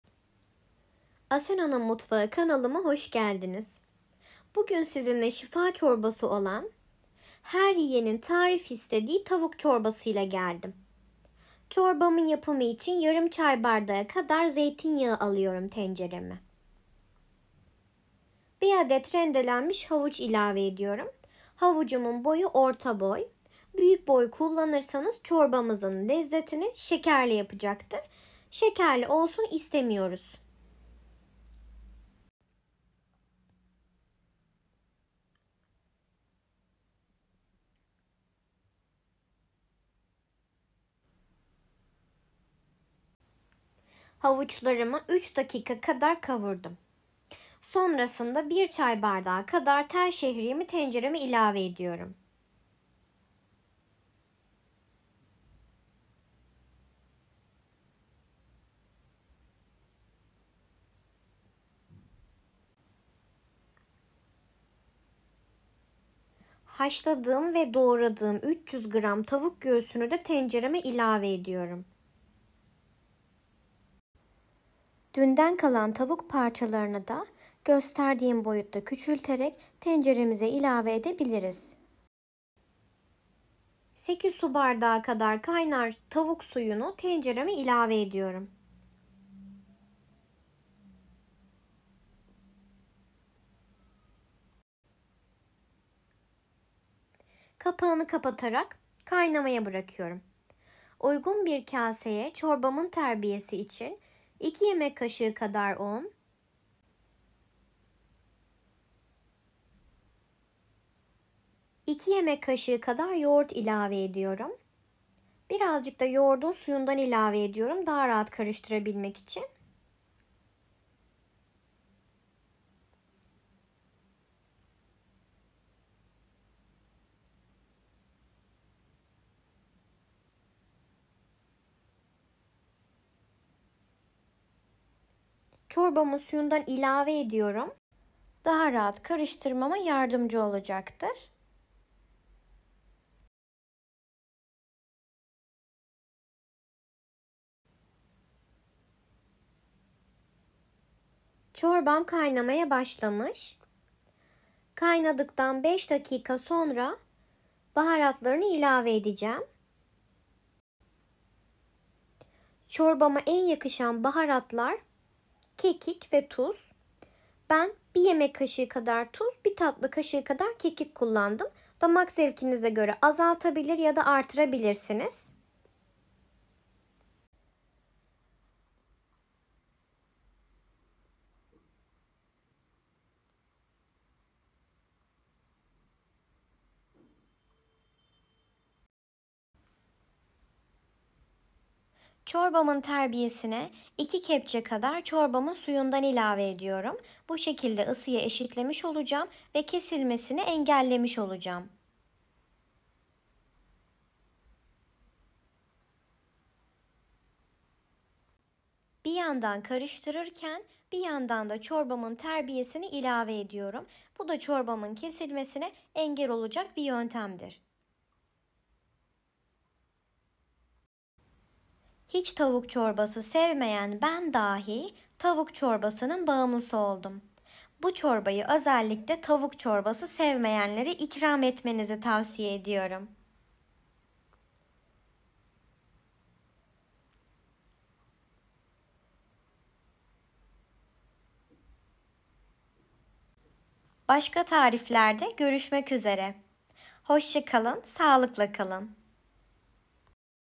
✅ Tavuk çorbasını bir de böyle deneyin 😋şifa çorbası 🔝🤌🏻
malzemeler
Tavuk göğsü
Yarım çay bardağı zeytin yağı (67 gr)
1 çay bardağı tel şehriye ( 45 gr )
1 adet orta boy  havuç
8 su bardağı tavuk suyu (1600 ml )
2 yemek kaşığı yoğurt (45 gr )
2 yemek kaşığı un (16 gr )
kekik, tuz